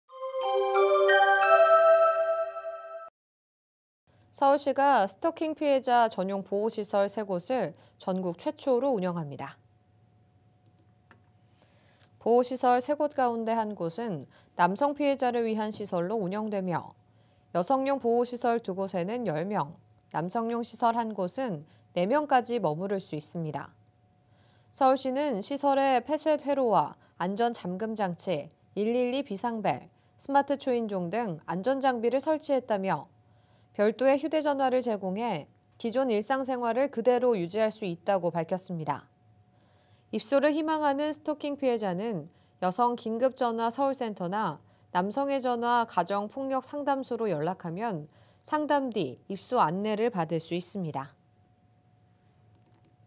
[서울뉴스]서울시, 스토킹 피해자 보호시설 운영
서울시가 스토킹 피해자 전용 보호시설
3곳을 전국 최초로 운영합니다.

보호시설 3곳 가운데 한 곳은 남성 피해자를 위한 시설로 운영되며
여성용 보호시설 2곳에는 10명,
남성용 시설 1곳은 4명까지 머무를 수 있습니다.

서울시는 "시설에 폐쇄회로와 안전잠금장치, 112비상벨, 스마트초인종 등
안전 장비를 설치했다"며 "별도의 휴대전화를 제공해
기존 일상생활을 그대로 유지할 수 있다"고 밝혔습니다.

입소를 희망하는 스토킹 피해자는 '여성긴급전화 서울센터'나
'남성의전화 가정폭력상담소'로 연락하면
상담 뒤 입소 안내를 받을 수 있습니다.